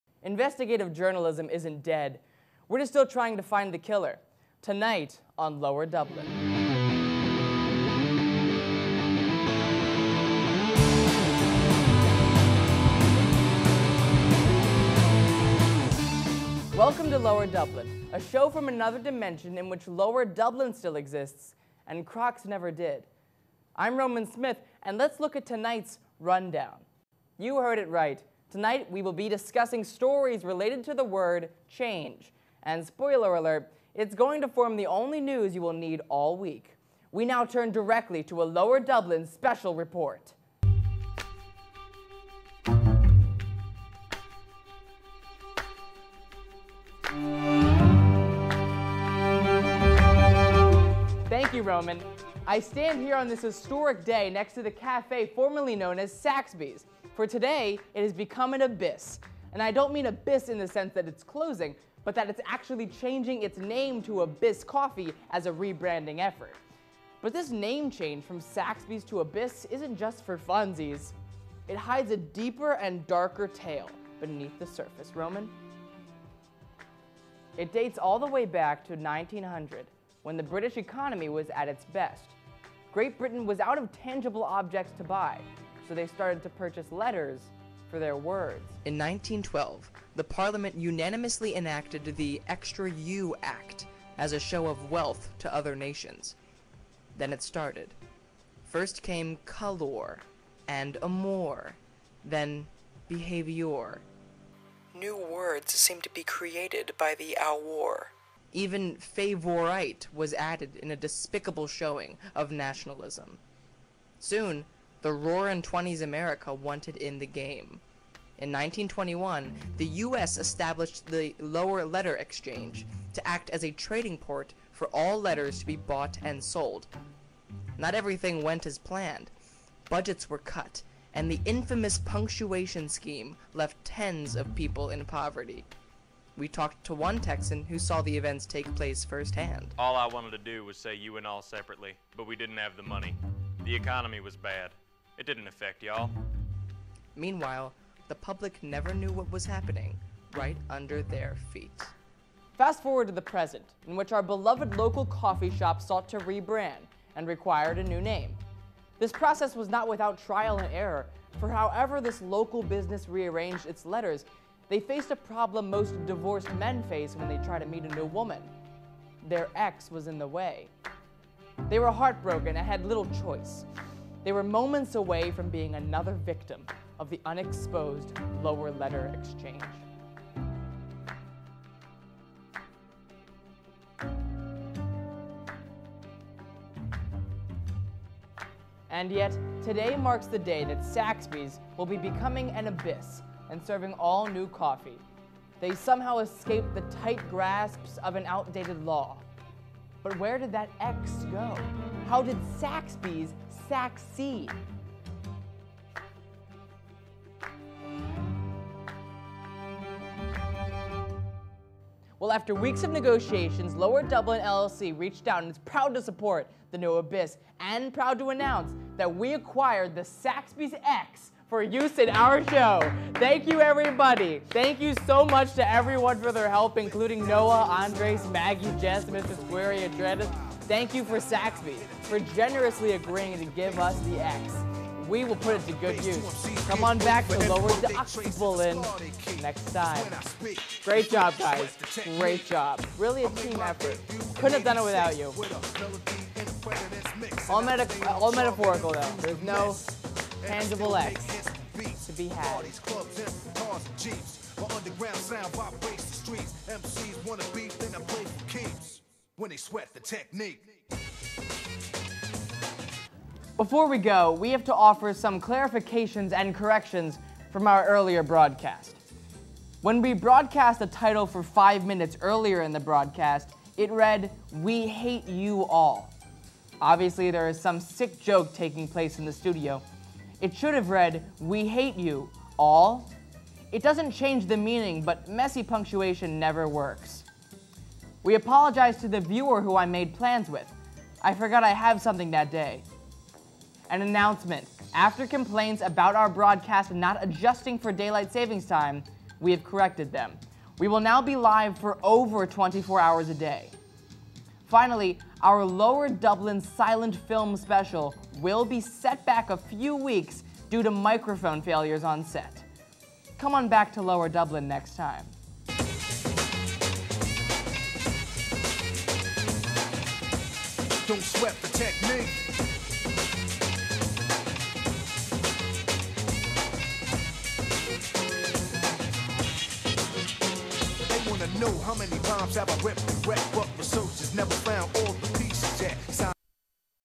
Lower Dublin - Saxbys becomes Abyss
On Tonight's Episode of Lower Dublin- We dive head first into the enigma of a local coffee shop changing its name, and what it really means.